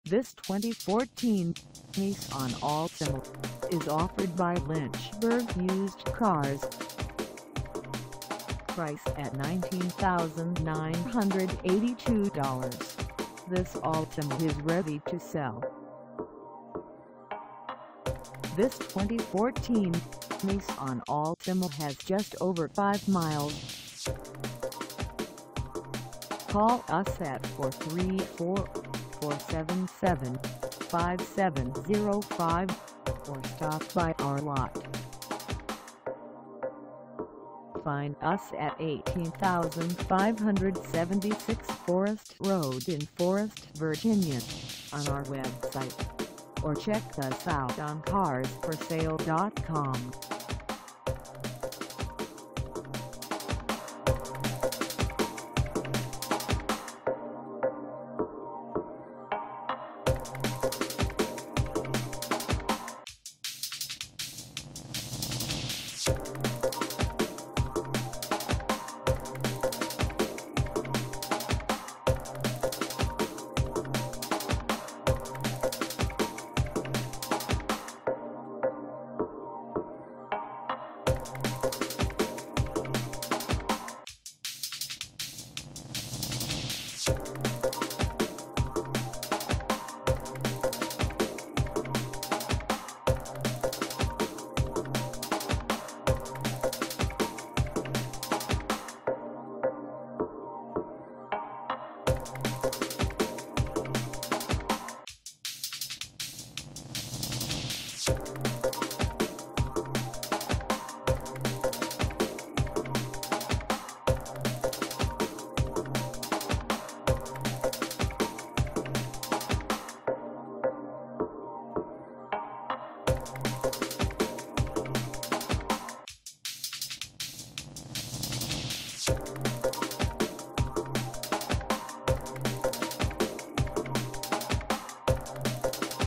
2014 Nissan Altima S - for sale in Forest, VA 24551
Lynchburg Used Cars
18576 FOREST RD
in Forest, VA 24551

Come test drive this 2014 Nissan Altima S for sale in Forest, VA.

[L92] FLOOR MATS W/O TRUNK MAT (4 PIECE),Steel Spare Wheel,Compact Spare Tire Mounted Inside Under Cargo,Clearcoat Paint,Body-Colored Front Bumper,Body-Colored Rear Bumper,Chrome Side Windows Trim and Black Front Windshield Trim,Chrome Door Handles,Fixed Rear Window w/Defroster,Light Tinted Glass,Speed Sensitive Variable Intermittent Wipers,Front Windshield -inc: Sun Visor Strip,Galvanized Steel/Aluminum Panels,Black Grille w/Chrome Surround,Trunk Rear Cargo Access,Manual Tilt/Telescoping Steering Column,Gauges -inc: Speedometer Odometer Engine Coolant Temp Tachometer Trip Odometer and Trip Computer,Power Rear Windows,Front Cupholder,Rear Cupholder,Front Cigar Lighter(s),Ashtray,Valet Function,Remote Releases -Inc: Power Trunk/Hatch and Mechanical Fuel,Illuminated Locking Glove Box,Driver Foot Rest,Full Cloth Headliner,Full Floor Console w/Covered Storage Mini Overhead Console w/Storage and 2 12V DC Power Outlets,Front And Rear Map Lights,Full Carpet Floor Covering,Carpet Floor Trim and Carpet Trunk Lid/Rear Cargo Door Trim,Cargo Area Concealed Storage,Cargo Space Lights,Driver / Passenger And Rear Door Bins,Delayed Accessory Power,Power Door Locks w/Autolock Feature,Systems Monitor,Trip Computer,Outside Temp Gauge,Analog Display,Front Center Armrest w/Storage and Rear Center Armrest,Perimeter Alarm,Engine Immobilizer,2 12V DC Power Outlets,Engine: 2.5L DOHC 16-Valve I-4,Transmission: Xtronic CVT (Continuously Variable),Front-Wheel Drive,4.83 Axle Ratio,550CCA Maintenance-Free Battery w/Run Down Protection,110 Amp Alternator,Gas-Pressurized Shock Absorbers,Front And Rear Anti-Roll Bars,Electric Power-Assist Speed-Sensing Steering,18 Gal. Fuel Tank,Quasi-Dual Stainless Steel Exhaust w/Chrome Tailpipe Finisher,Strut Front Suspension w/Coil Springs,Multi-Link Rear Suspension w/Coil Springs,4-Wheel Disc Brakes w/4-Wheel ABS Front Vented Discs Panic Brake Assist and Hill Hold Control,Electronic Stability Control (ESC),ABS And Driveline Traction Control,Side Impact Beams,Dual Stage Driver And Passenger Seat-Mounted Side Airbags,Dual Stage Driver And Passenger Front Airbags,Curtain 1st And 2nd Row Airbags,Airbag Occupancy Sensor,Rear Child Safety Locks,Outboard Front Lap And Shoulder Safety Belts -inc: Rear Center 3 Point Height Adjusters and Pretensioners,Bluetooth